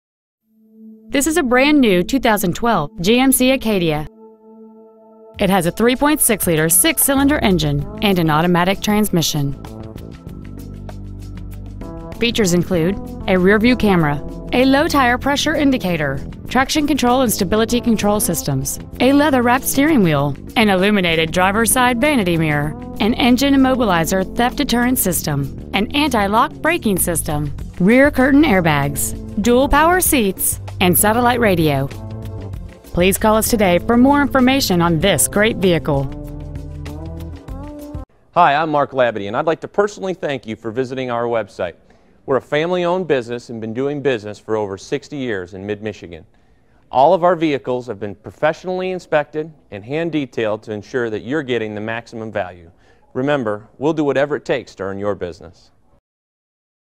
2012 GMC Acadia Midland MI
Year : 2012
 Make : GMC
 Model : Acadia
 Engine : 3.6L 6-Cylinder
 Trans . : automatic
 Exterior : Crystal Red Tintcoat
 Miles : 1
 Interior : Ebony
 Stock : G135910

 2012 GMC Acadia  MI